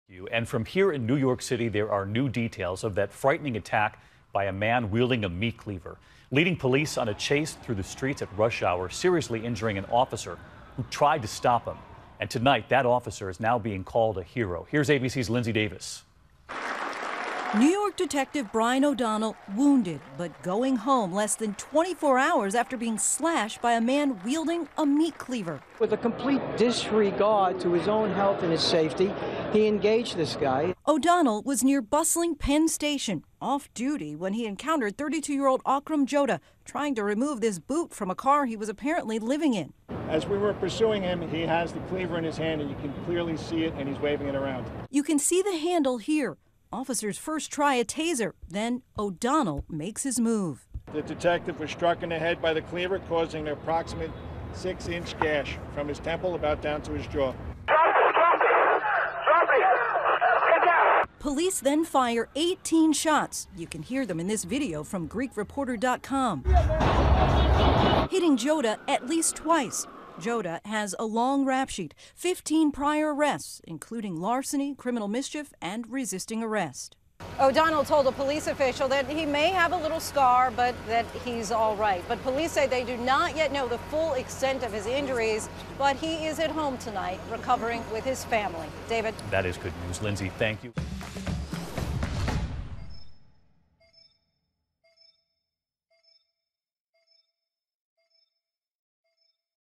Heroic Police Officer Slashed by Man Wielding Meat Cleaver Released From Hospital
Brian O'Donnell was off duty when he was allegedly attacked by 32-year-old Akram Joudeh.